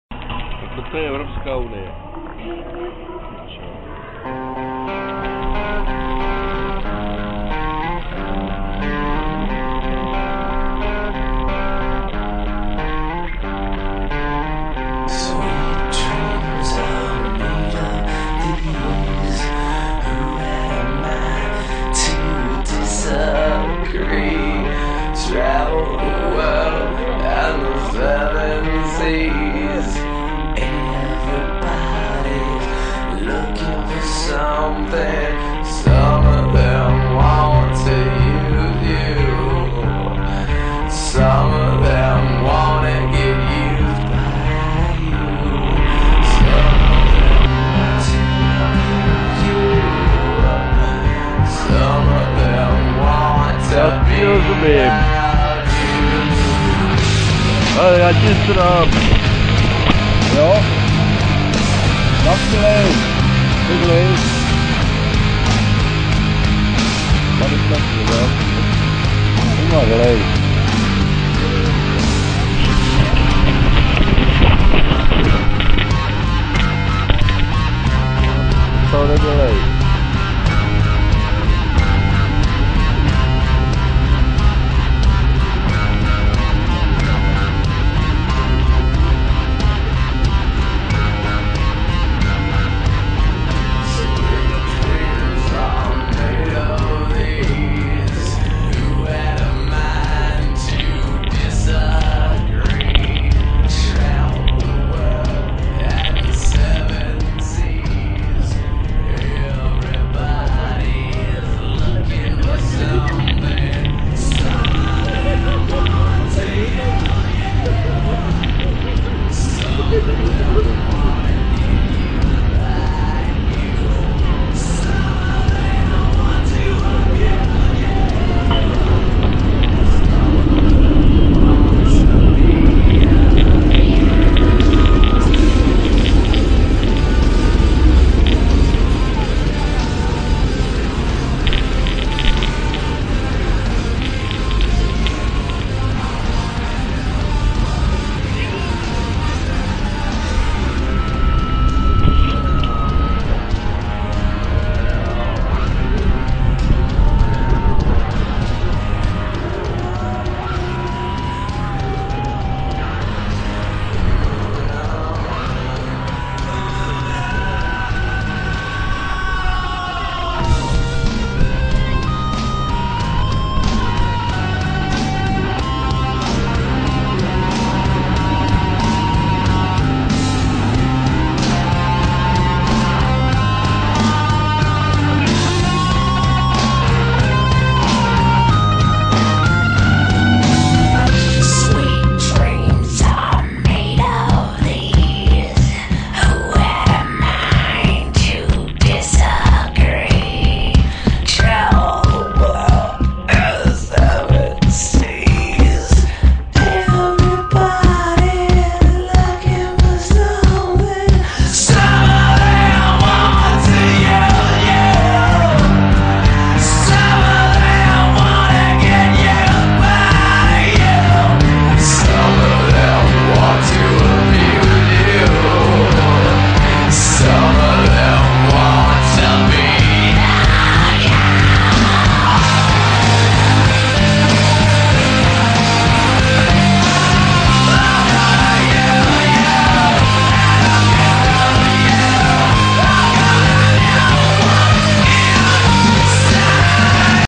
žebráci junioři v Rumunsku/EU/
čiperná partička v Oradeii,někdy dost vlezlí.
Nahráno špionážní tužkou za 350kaček.